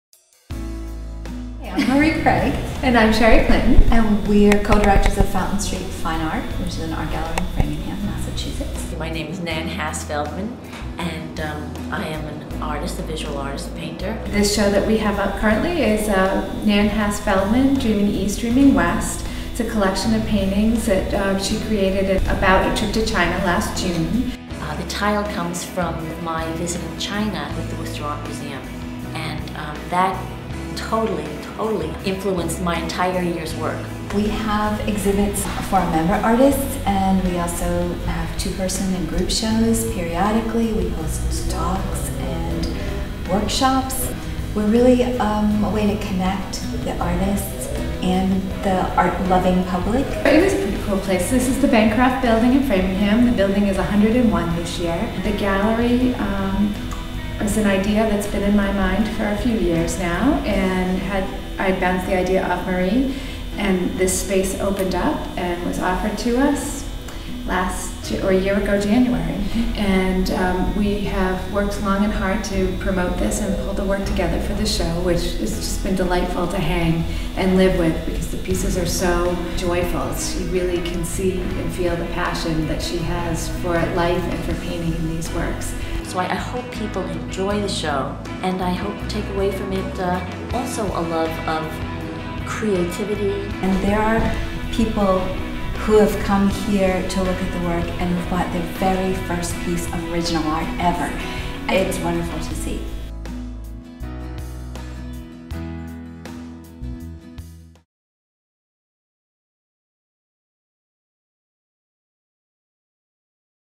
Fountain Street Fine Art :Dreaming East, Dreaming West
Nan Hass Feldman's solo show Dreaming East, Dreaming West at Fountain Street FIne Art, March 2012